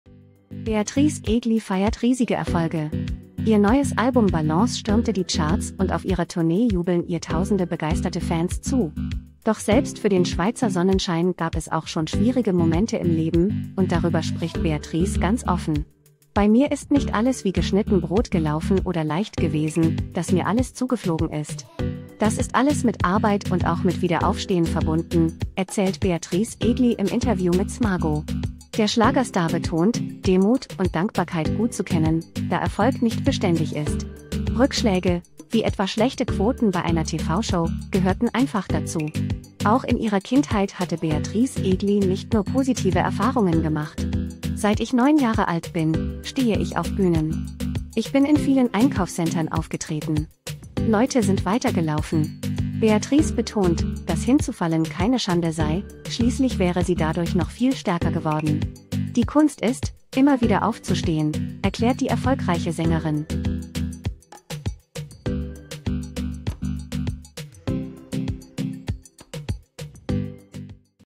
Beatrice Egli offen wie nie übers Scheitern: „Hinzufallen keine Schande“ • PROMIPOOL
Mit einem emotionalen Statement offenbart Beatrice Egli, wie sie mit Rückschlägen in der Vergangenheit umgehen musste. Für die Schweizerin gab es durchaus schwierige Zeiten, die sie aber überwinden konnte.